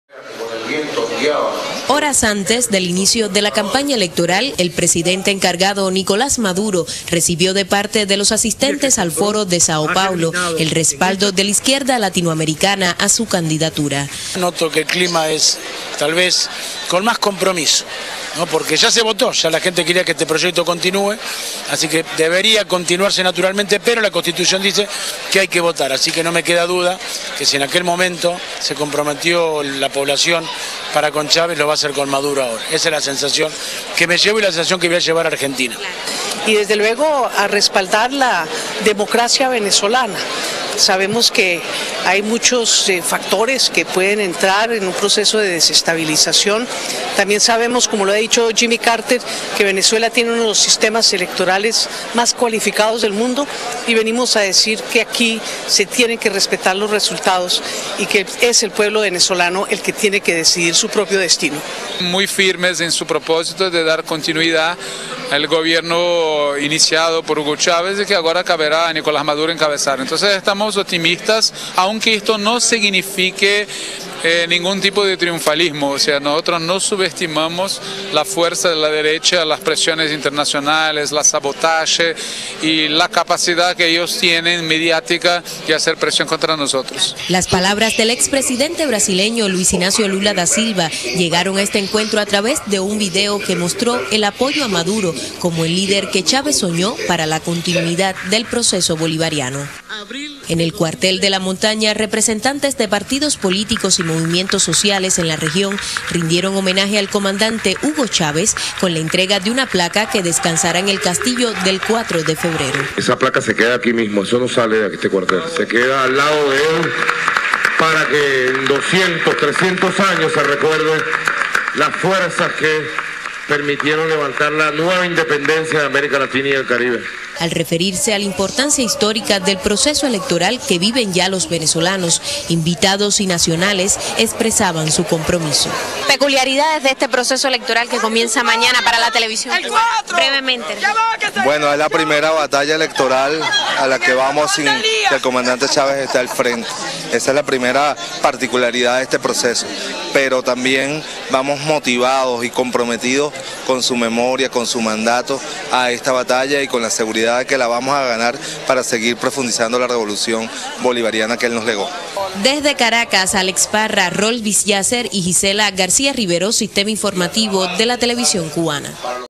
Venezuela: Izquierda latinoamericana respalda candidatura de Nicolás Maduro
Una reunión extraordinaria del Foro de Sao Paulo se celebró en el Cuartel de la Montaña, donde descansan los restos de Hugo Chávez Frías. Al encuentro con la izquierda latinoamericana asistió el Presidente Encargado de Venezuela, Nicolás Maduro, para agradecer el apoyo a su candidatura por las fuerzas más progresistas del continente.